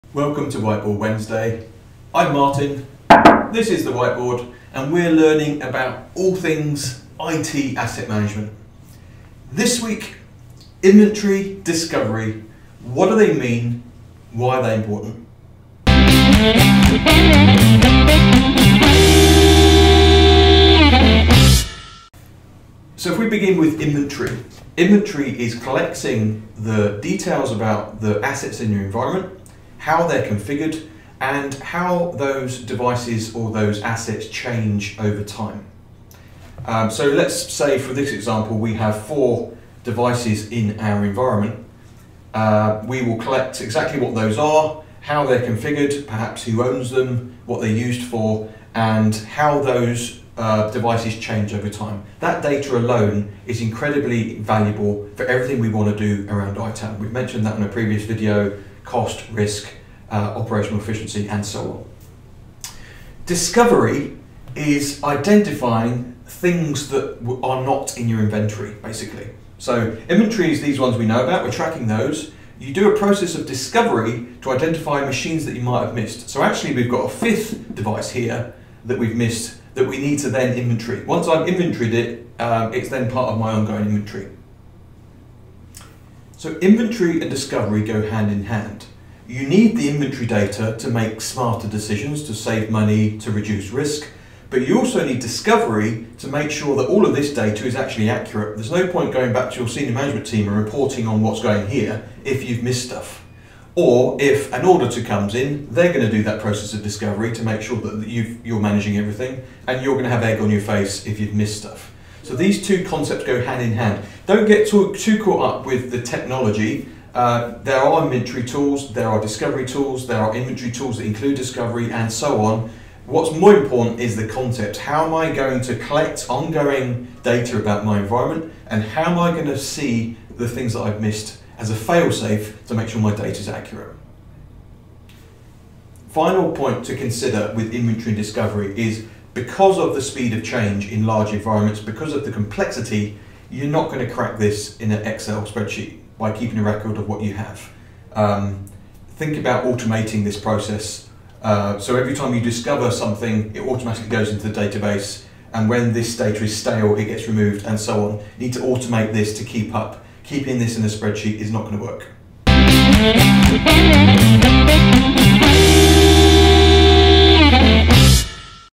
What's the difference between IT Inventory and Discovery?
Whiteboard Wednesday Episode 8: What's the difference between IT Inventory and Discovery?

and let us know what other topics you would like covered on the White Board!